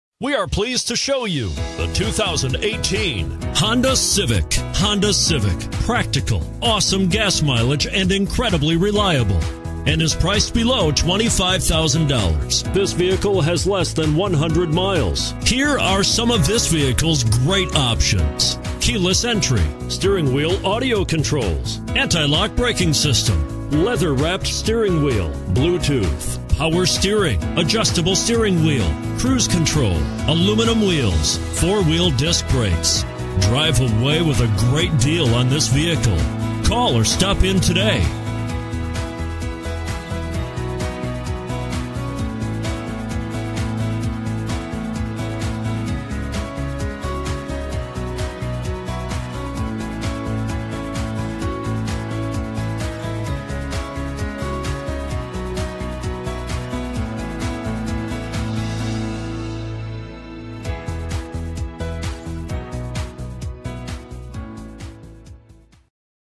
2018 Honda Civic Sedan HN182163
2018 HONDA CIVIC SEDAN Nanuet, NY
Stock# HN182163

Heated Leather Seats, Moonroof, Aluminum Wheels, Turbo Charged. EX-L trim, Crystal Black Pearl exterior and Black interior. FUEL EFFICIENT 42 MPG Hwy/32 MPG City! Warranty 5 yrs/60k Miles - Drivetrain Warranty; SEE MORE!

Leather Seats, Sunroof, Heated Driver Seat Honda EX-L with Crystal Black Pearl exterior and Black interior features a 4 Cylinder Engine with 174 HP at 6000 RPM*. Rear Spoiler, MP3 Player, Keyless Entry, Steering Wheel Controls, Child Safety Locks, Heated Mirrors, Electronic Stability Control.

EXPERTS CONCLUDE
Edmunds.com's review says  All around, the Civic's driving performance is top-notch, with strong brakes and accurate steering. .